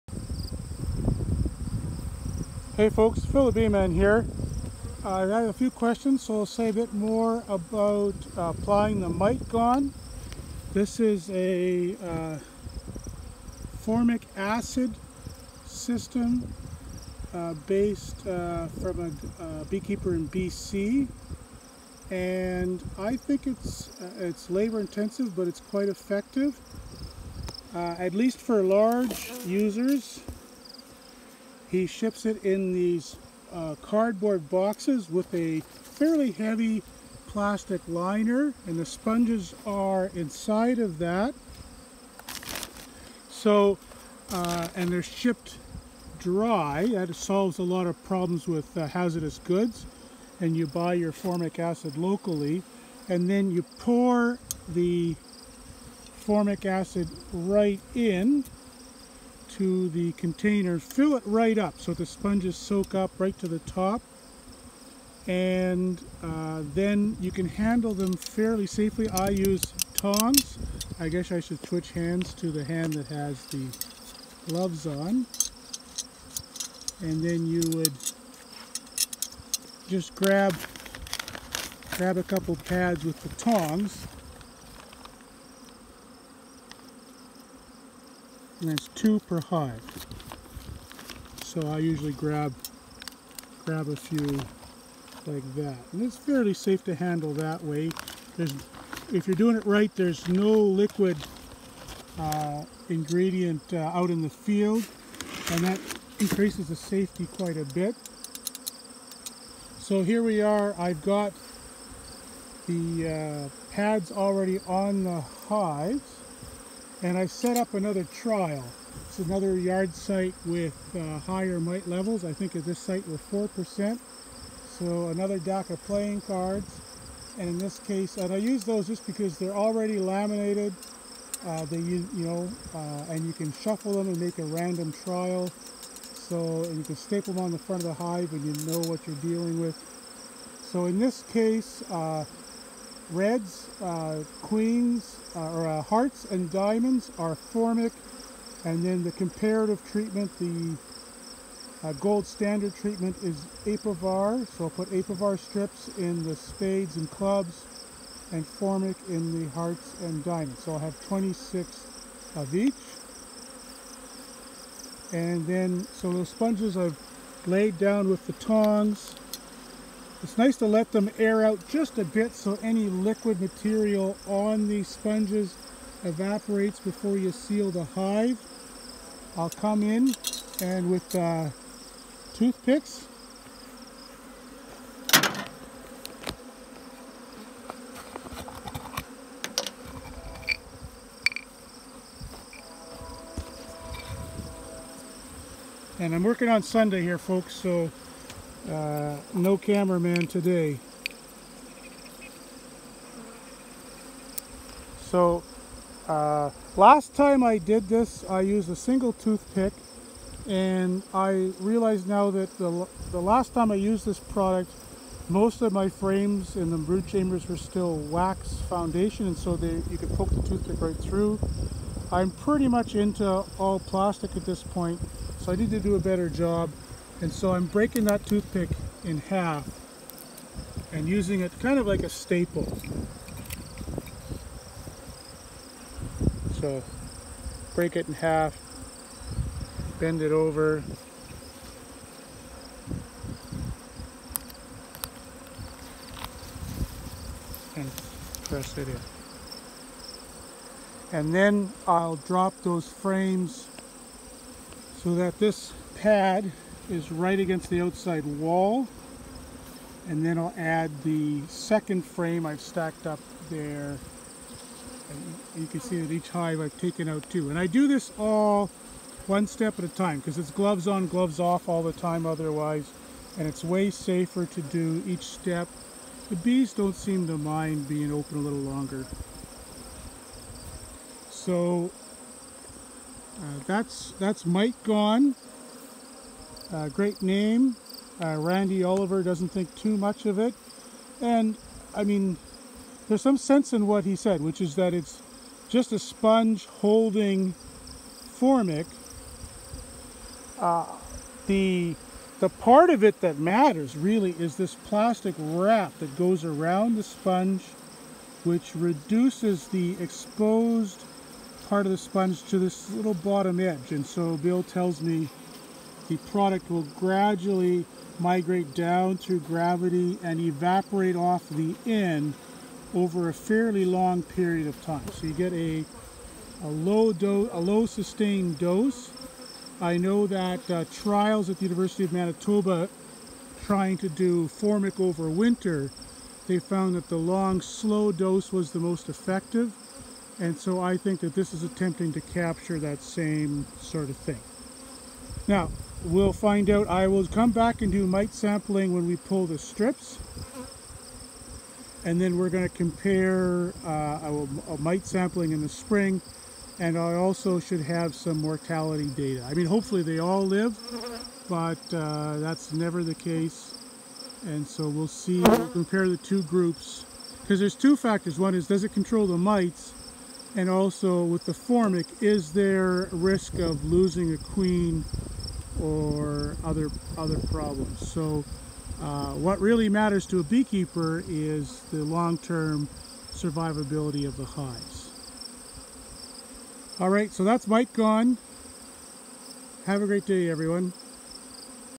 MiteGone application details
A bit more detail about how I use the Mitegone pads to apply formic acid for Varroa mite control.  I've also set these up in a trial vs apivar.  For future reference:  Hearts and diamonds are formic, Spades and Clubs are apivar.  Cards are Disney Star Wars Themed.